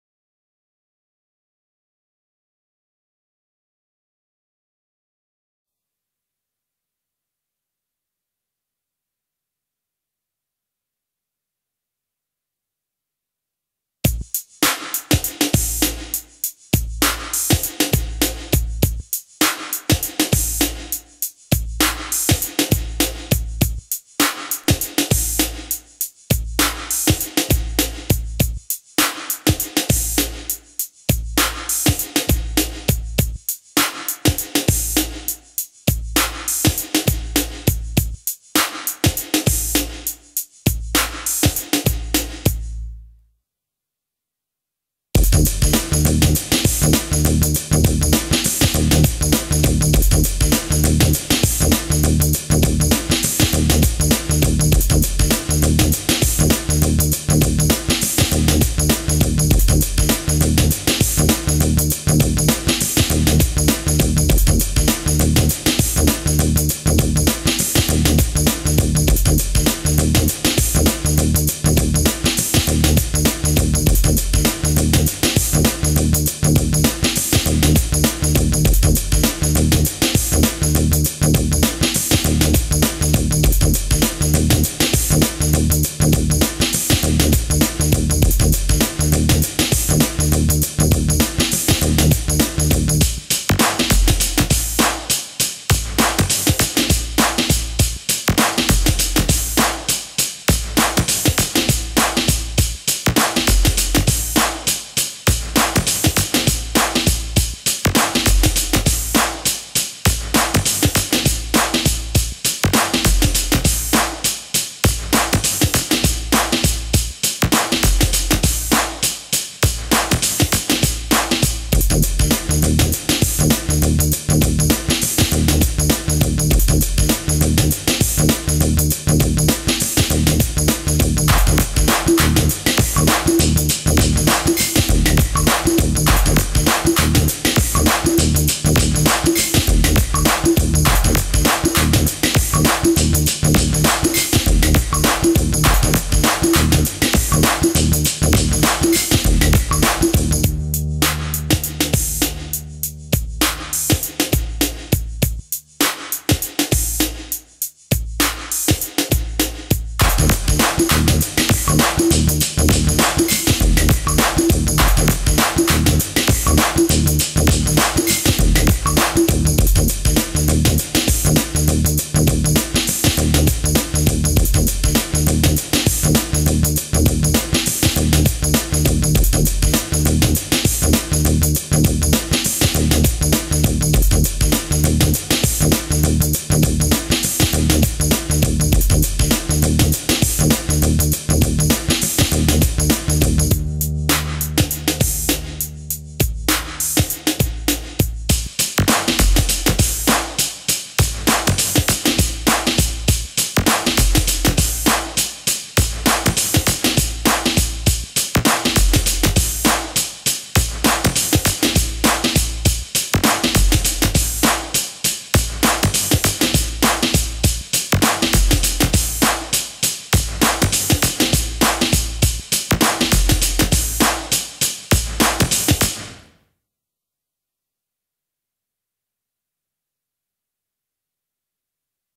660 Breaks to the Beats Part 2
Use 1080p for best sound. Song by Syntza. Efx by Roland SRV-2000. Instruments used: Boss DR-660. Triggered by Akai MPC-1000.